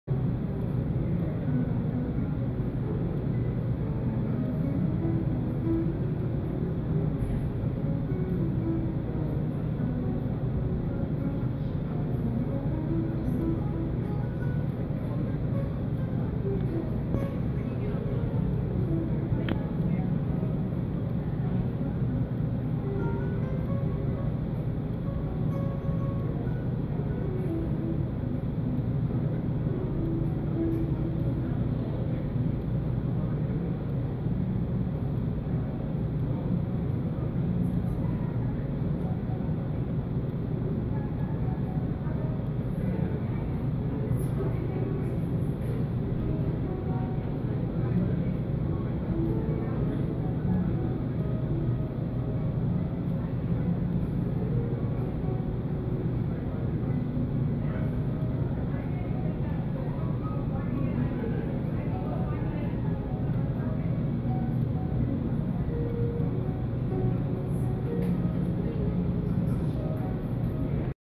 Glass cleaning of a high rise office building in the GTA
High rise window cleaner in the GTA, Ontario, Canada